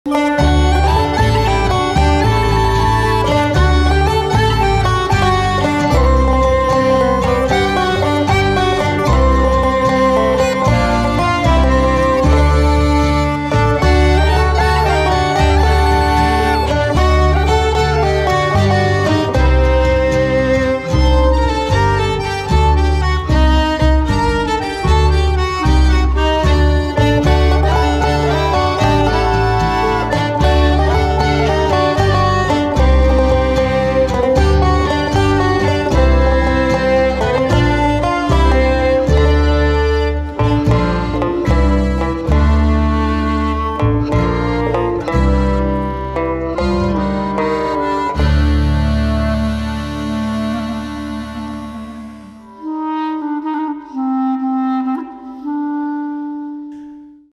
Mirk musicians filming in Red Bank (Mcr Jewish Museum) - trailer
A trailer - specifically highlighting the Red Bank/Manchester Jewish Museum connection - of the filming of musicians for the Amid the Mirk Over the Irk (klezmerirish.com) project.